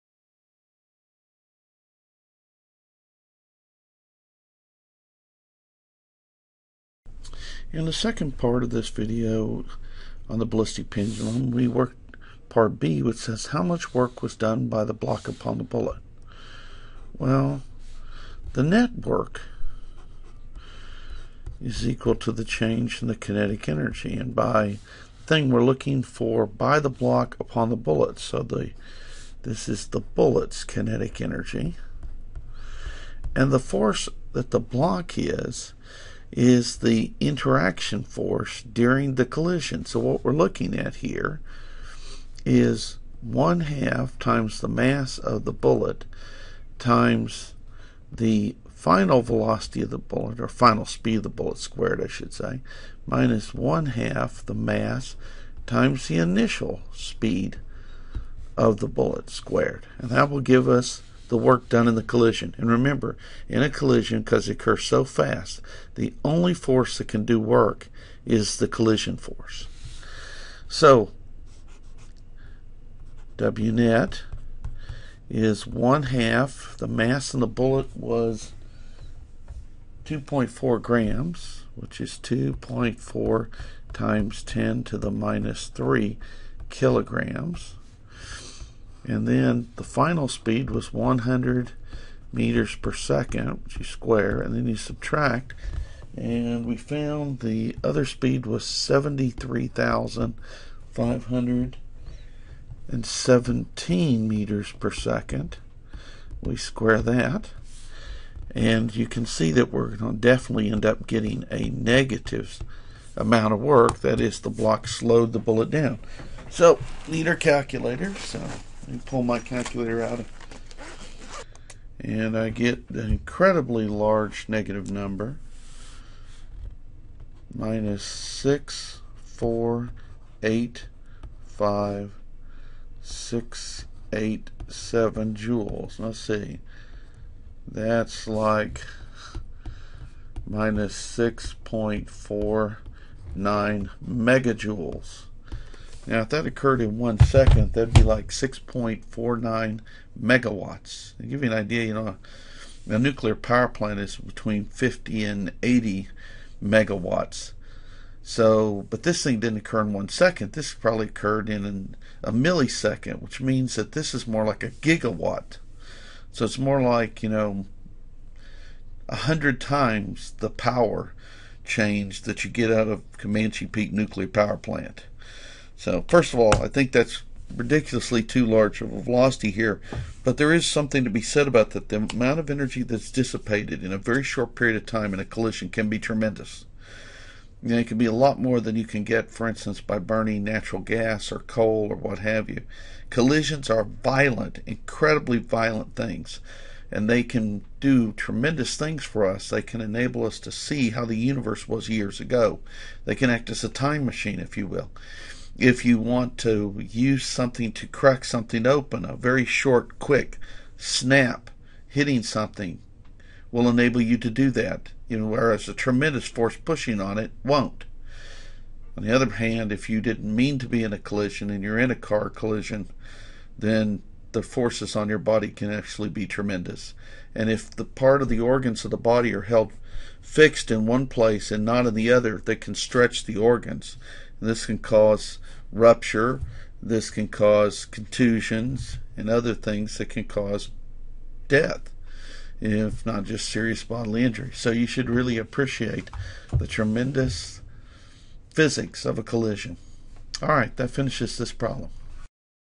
Balistic Pendulum Problem Part 2
2nd part of the ballistic pendulum looking at how one can calculate the work by the collision force upon the bullet without knowing the exact nature of the force.